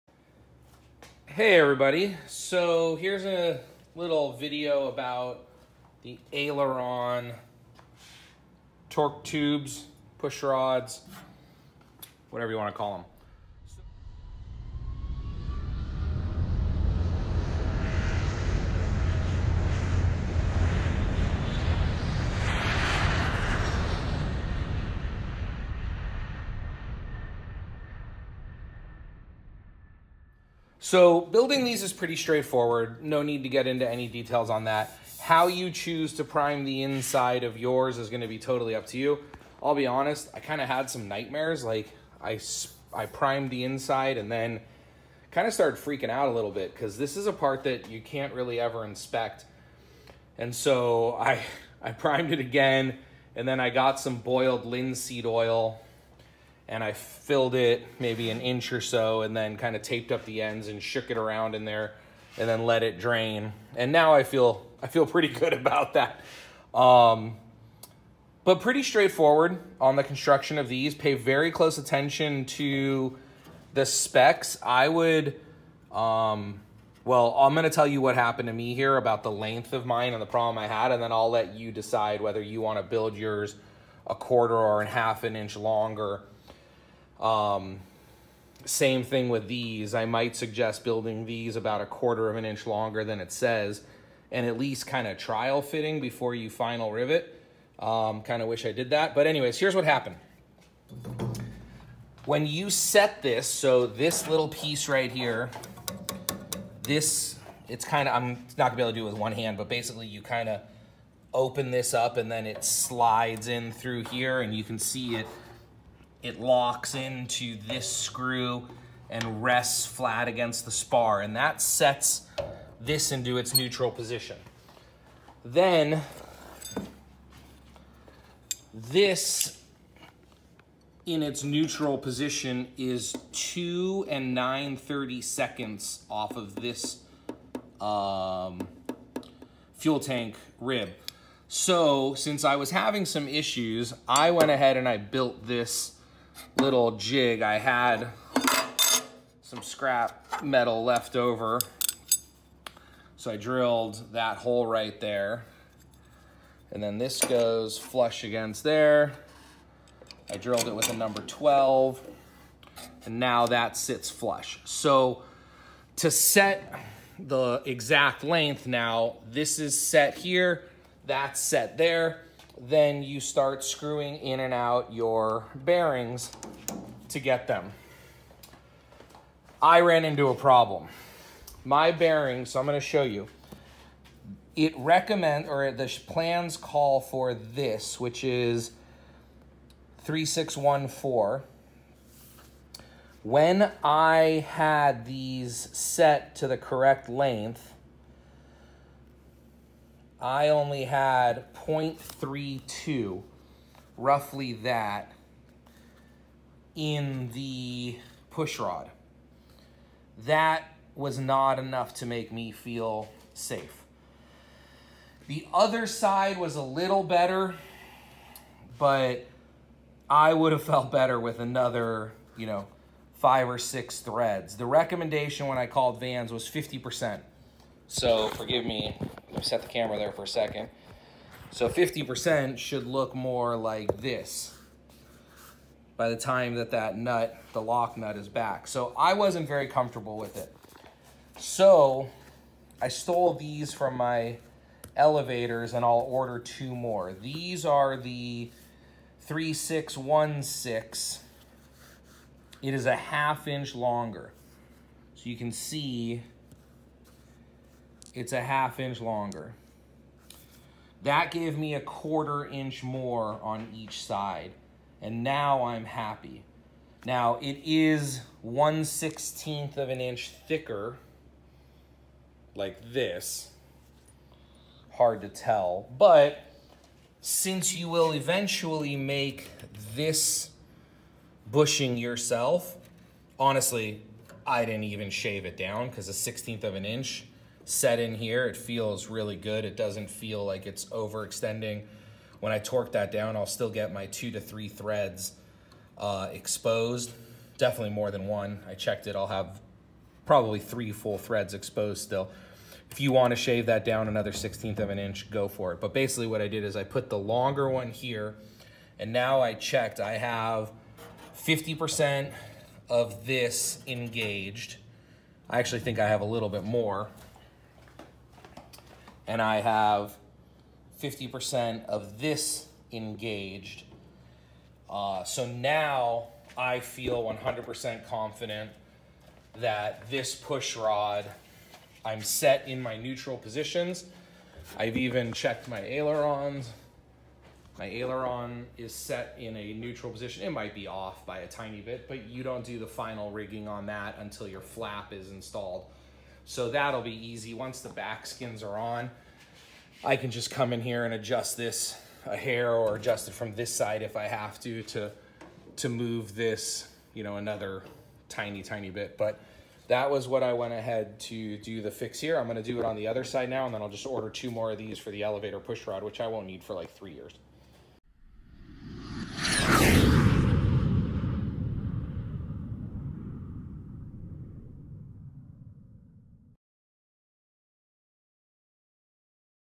Aileron Torque Tubes Vans Aircraft RV10 Build
Building the aileron torque tubes and aileron pushrods was pretty straightforward, but you have to make sure you cut them to the EXACT right size.  In fact, I would recommend cutting them about 1/2 an inch longer than the plans call for.

If you decide to order a Van’s Aircraft Kit, please let them know that Ryan sent you and use my builder number #41983

Be Amazing,
Pilot Rhino